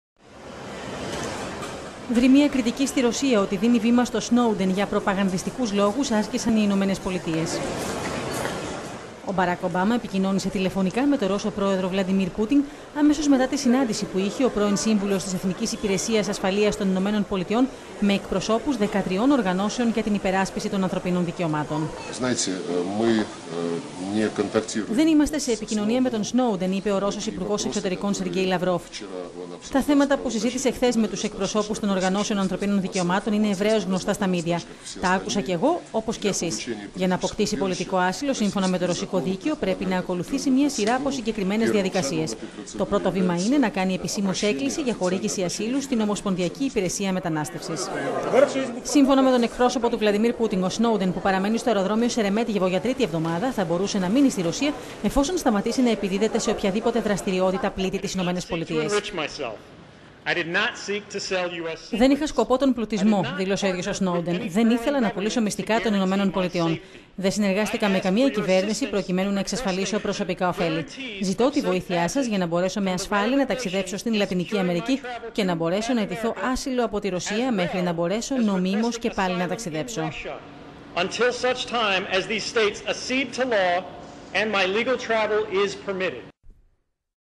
Ένταση στις σχέσεις ΗΠΑ- Ρωσίας προκαλεί η πρώτη εμφάνιση Σνόουντεν
Δριμεία κριτική στην Ρωσία ότι δίνει βήμα στον...

Δριμεία κριτική στην Ρωσία ότι δίνει βήμα στον Σνόουντεν, για προπαγανδιστικούς λόγους άσκησαν οι ΗΠΑ.

Ο ίδιος ο πρόεδρος των ΗΠΑ Μπαράκ Ομπάμα επικοινώνησε τηλεφωνικά με τον Ρώσο πρόεδρο Βλαντιμίρ Πούτιν, αμέσως μετά την συνάντηση που είχε ο πρώην σύμβουλος της Εθνικής Υπηρεσίας Ασφαλείας των ΗΠΑ , με εκπροσώπους, 13 οργανώσεων για την υπεράσπιση των ανθρωπίνων δικαιωμάτων.

Ο Ρώσος υπουργός Εξωτερικών Σεργκέι Λαβρόφ, διευκρίνισε την επίσημη θέση του Κρεμλίνου.

" Δεν είμαστε σε επικοινωνία με τον Σνόουντεν. Τα θέματα που συζήτησε χθες με τους εκπροσώπους των οργανώσεων  ανθρωπίνων δικαιωμάτων, είναι ευρέως γνωστά στα μίντια. Τα άκουσα όπως και όλοι εσείς. Για να αποκτήσει πολιτικό άσυλο σύμφωνα με το ρωσικό δίκαιο, πρέπει να ακολουθήσει μια σειρά από συγκεκριμένες διαδικασίες. Το πρώτο βήμα είναι να κάνει επισήμως έκκληση για χορήγηση ασύλου
στην ομοσπονδιακή υπηρεσία μετανάστευσης."

Σύμφωνα με τον εκπρόσωπο του Βλαντιμίρ Πούτιν, ο Σνόουντεν που παραμένει στο αεροδρόμιο Σερεμέτγιεβο για τρίτη εβδομάδα, θα μπορούσε να μείνει στην Ρωσία, εφόσον σταματήσει να επιδίδεται σε οποιαδήποτε δραστηριότητα πλήττει τις ΗΠΑ.